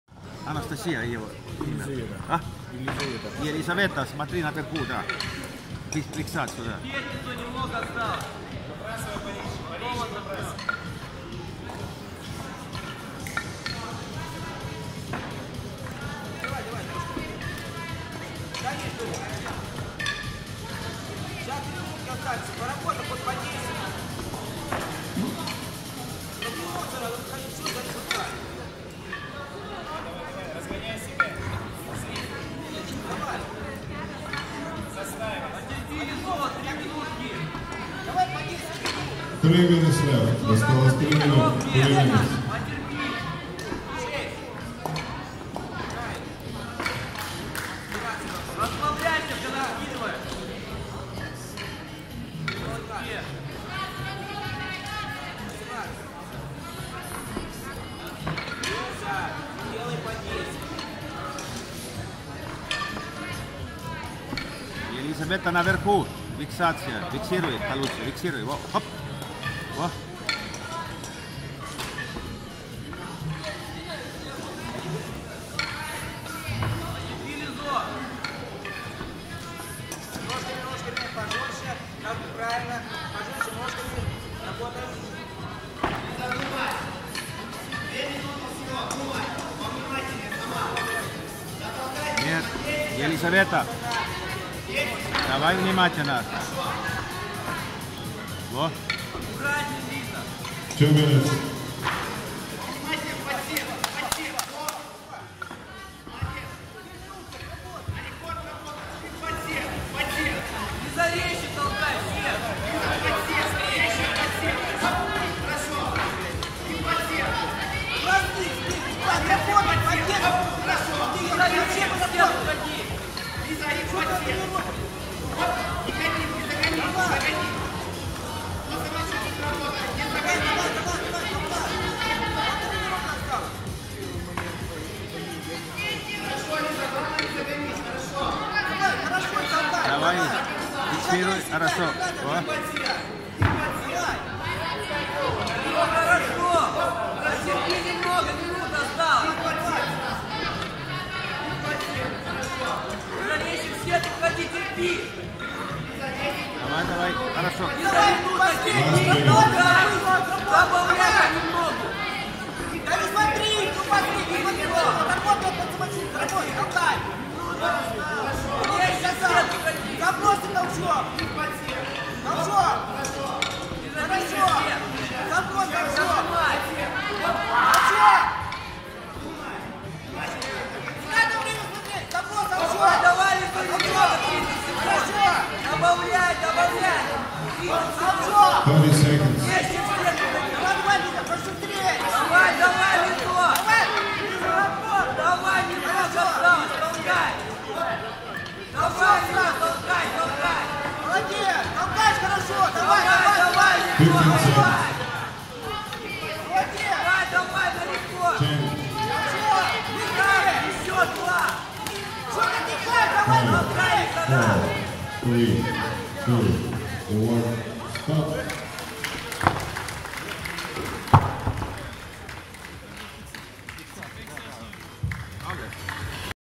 Елизавета Бенидзе. Толчок ДЦ 104 (16кг). Первенство Европы 2019, Париж
Первенство Европы среди юношей и девушек по гиревому спорту 28 июня-2 июля 2019, г. Париж (Франция)
Елизавета Бенидзе
Толчок длинным циклом 104 подъемов (ГИРИ 2*16 КГ).
весовая до 63 кг.